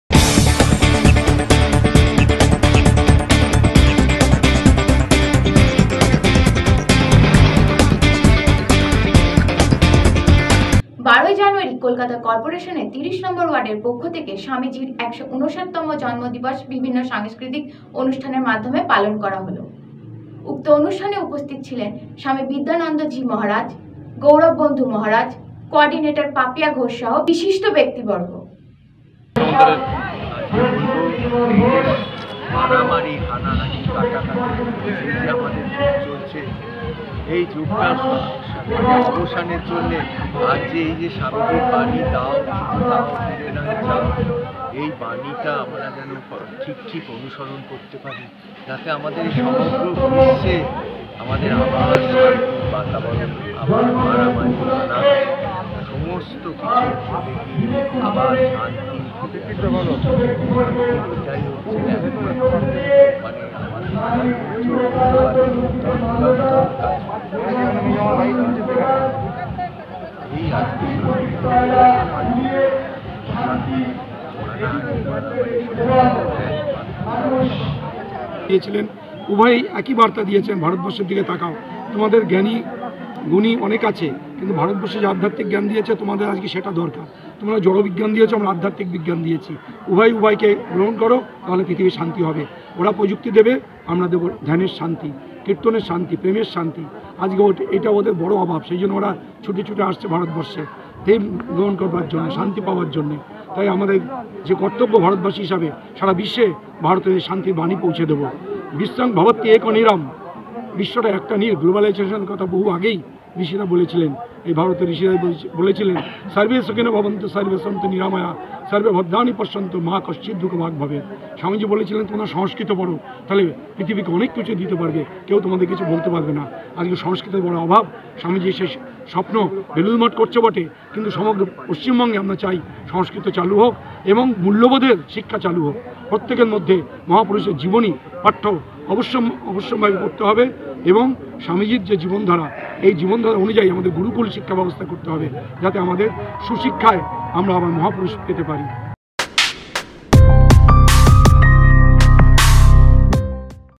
Celebration of Swamiji's birthday on behalf of Ward No. 30 of Calcutta Corporation.
Swamiji's 159th birth anniversary was celebrated on 12th January by Ward No. 30 of Calcutta Corporation through various cultural programs. Swami Vidyanandaji Maharaj, Gaurab Bandhu Maharaj, Co-ordinator Papia Ghosh and other eminent personalities were present on the occasion.

12 ই জানুয়ারি কলকাতা করপোরেশনের 30 নম্বর ওয়ার্ডের পক্ষ থেকে স্বামীজীর 159 তম জন্ম দিবস বিভিন্ন সাংস্কৃতিক অনুষ্ঠানের মাধ্যমে পালন করা হলো। উক্ত অনুষ্ঠানে উপস্থিত ছিলেন স্বামী বিদ্যানন্দজি মহারাজ, গৌরব বন্ধু মহারাজ, কো-অর্ডিনেটর পাপিয়া ঘোষ সহ বিশিষ্ট ব্যক্তিবর্গ।

Rajib Biswas
Back Eye Entertainment (CEO)